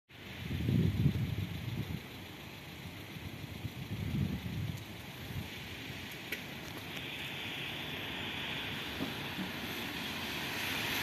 Пьяный водитель врезался в ограду Детского парка. "Фольксваген" загорелся
Видео очевидцев "ЧП Саратов/Прямой эфир"